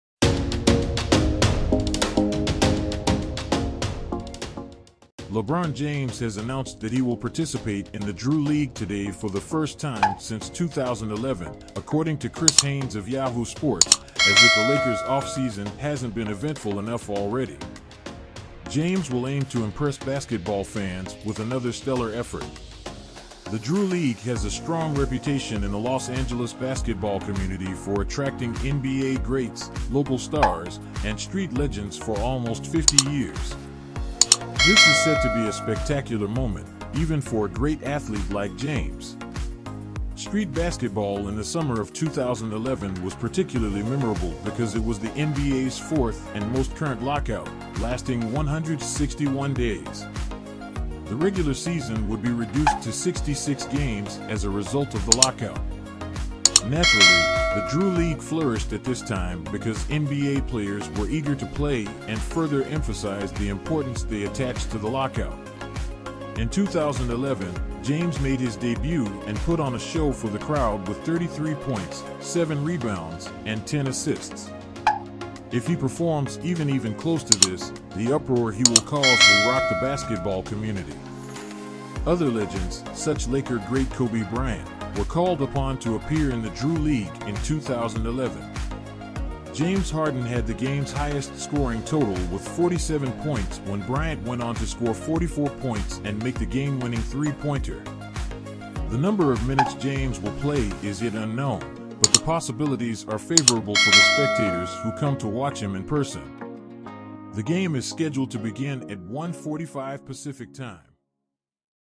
LeBron James will participate in a Drew League game.
LeBron James has announced that he will participate in the Drew League today for the first time since 2011, according to Chris Haynes of Yahoo Sports, as if the Lakers' offseason hasn't been eventful enough already. James will aim to impress basketball fans with another stellar effort.